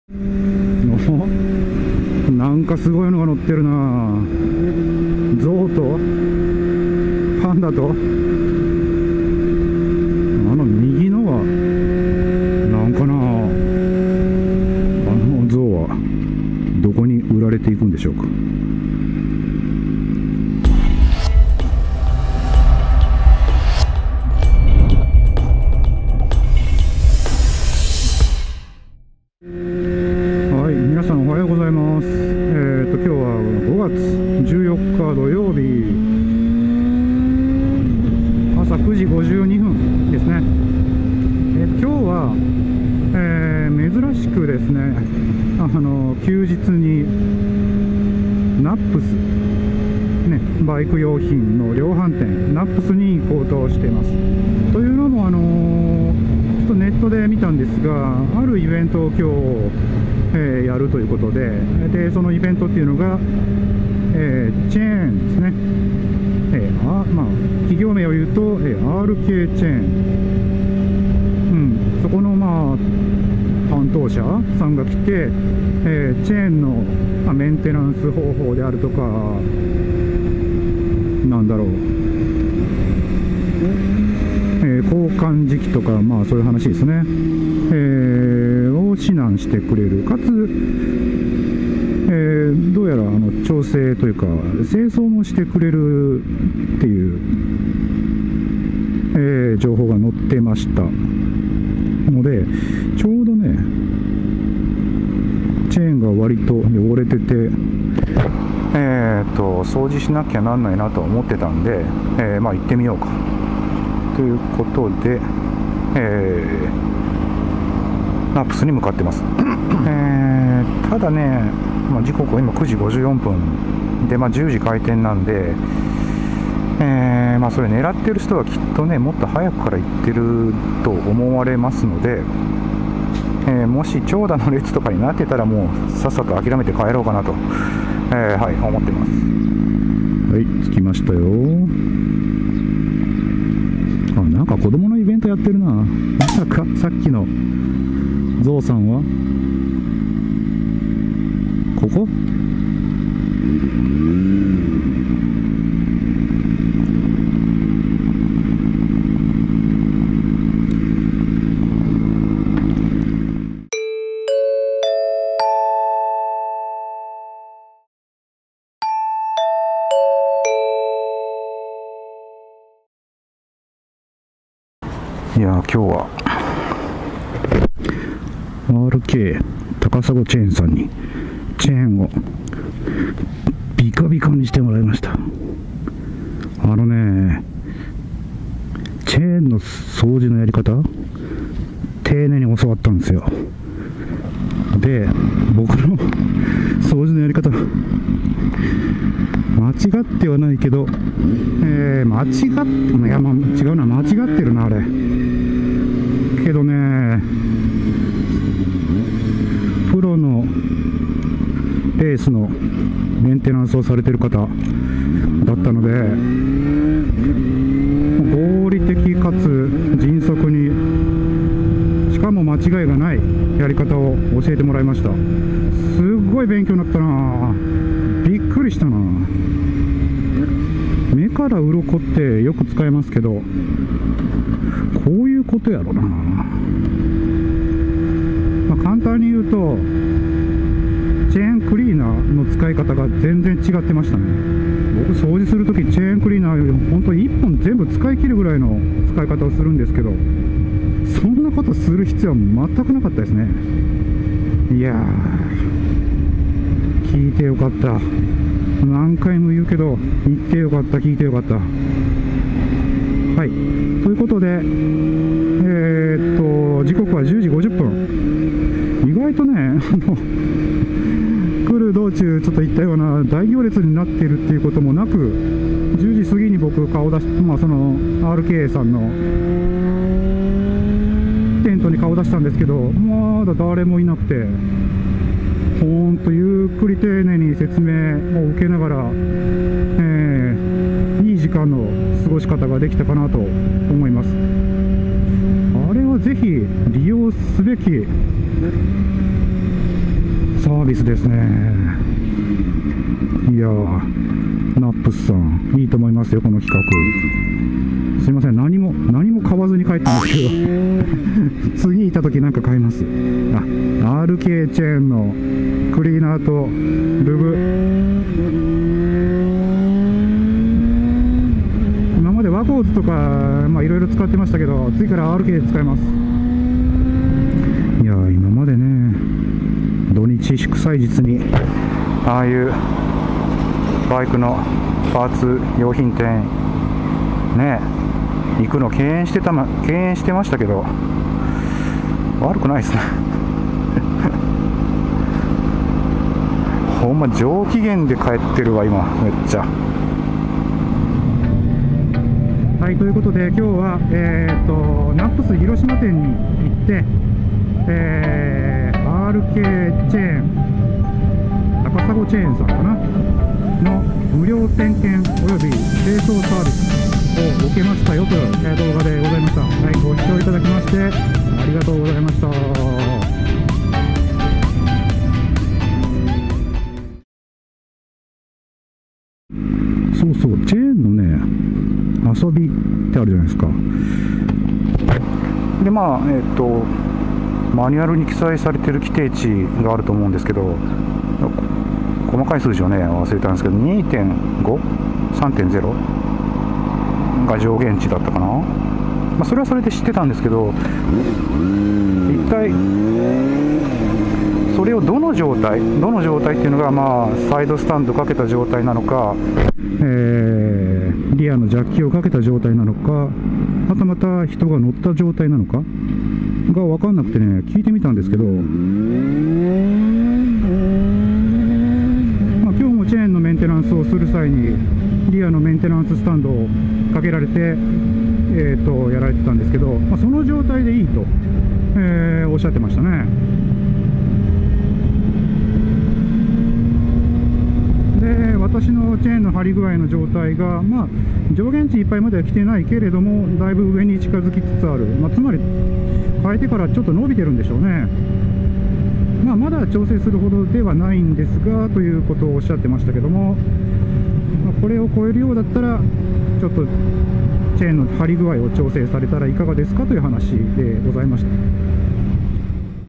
プロによるチェーンメンテをタダでやってもらったゾ！_122@HORNET250(ホーネット250)_モトブログ
2016/5/14、ナップス広島にRKエキセルの市川さんがお見えになり、チェーンの無料診断や メンテナンスのポイント説明・チェーンクリーニングまで行うイベントがありました。
ちょうど（？）チェーンも汚れているということもあり、行ってみることにしました。

/Camera
 HDR-AS30V

/Accessories
 Versatile Lightweight Open Skeleton Frame(AKA-SF1)
 Helmet Side Mount(VCT-HSM1)

/opening
 RAVENPRODESIGN VEGAS PRO TEMPLATES